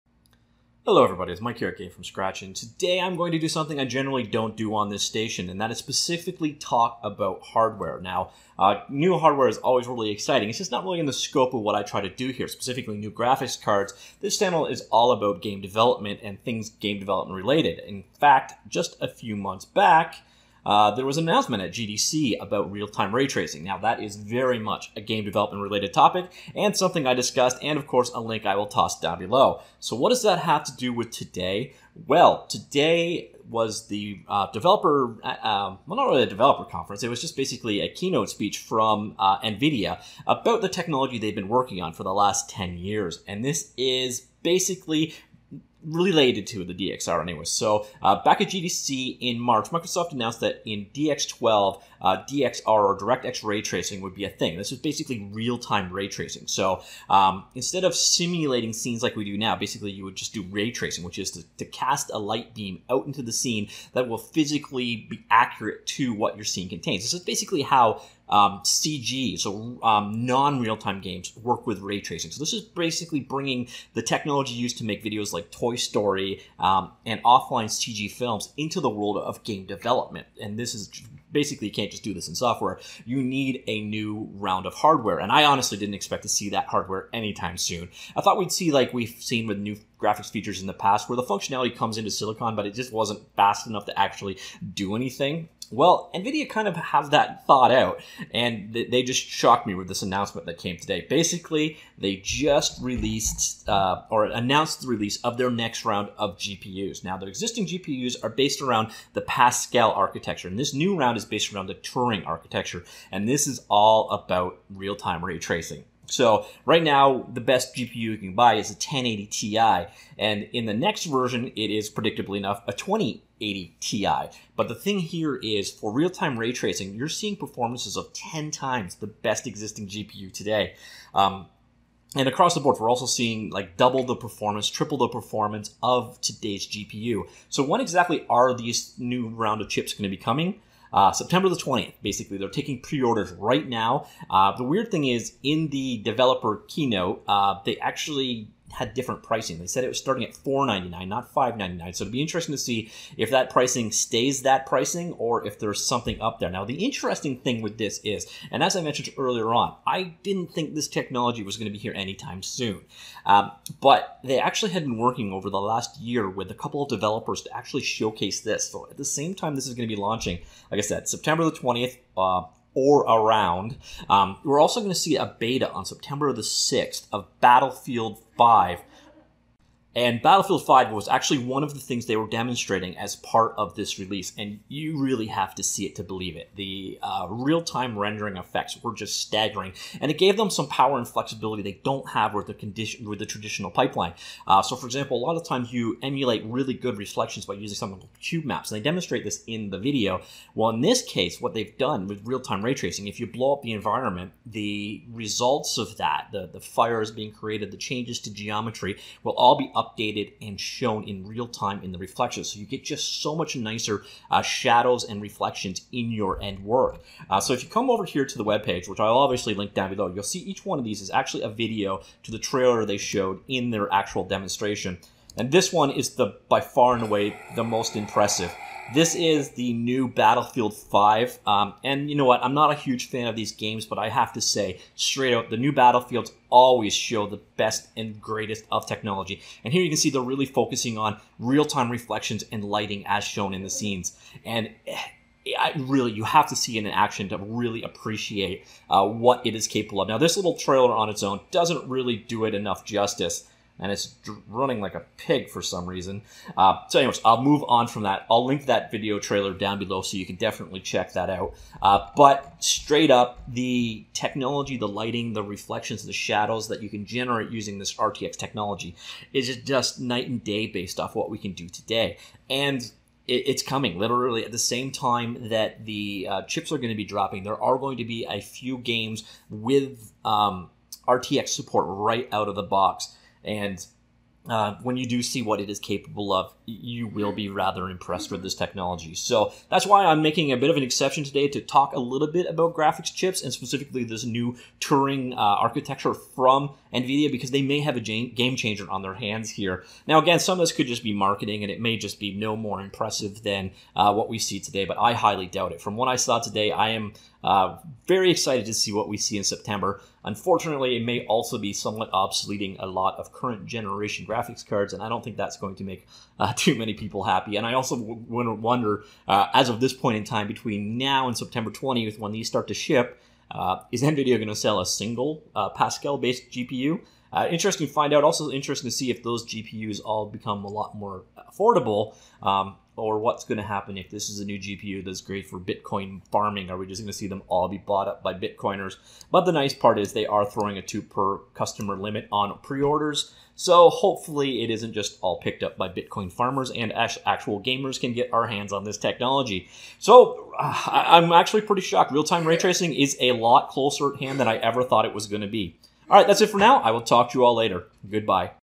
NVIDIA RTX -- Real Time Ray Tracing Arrives September!
Back in March, Microsoft announce DXR real time rendering for DX12 at GDC, and NVIDIA announced they would be supporting it in the form of RTX.  Well today, NVIDIA announced their next release of GPUs, the 2070/2080 and 2080ti will ship in September and RTX supported games such as Battlefield 5, Tomb Raider and Metro will ship along side them!